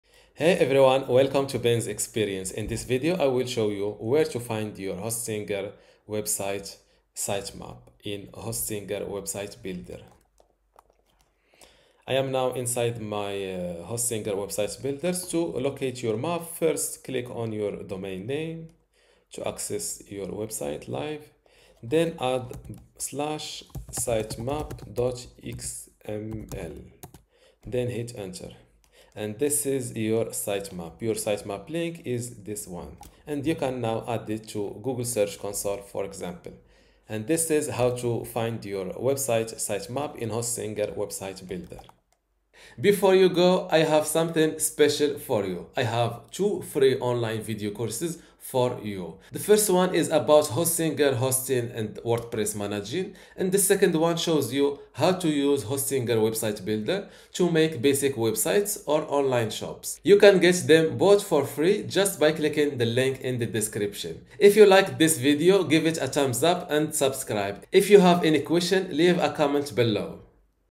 How to Find Your Sitemap in Hostinger Website Builder
In this video, I'll show you how to find your sitemap in Hostinger Website Builder. Understanding how to find your sitemap in Hostinger Website Builder is crucial for optimizing your website for search engines. To locate your sitemap, simply click on your domain name to access your website live, then add /sitemap.xml to the URL and hit enter. This will bring up your sitemap, which you can then add to tools like Google Search Console.

Knowing how to find your sitemap in Hostinger Website Builder ensures your website is properly indexed, helping you attract more visitors. Don't miss out on this essential step for your website's SEO!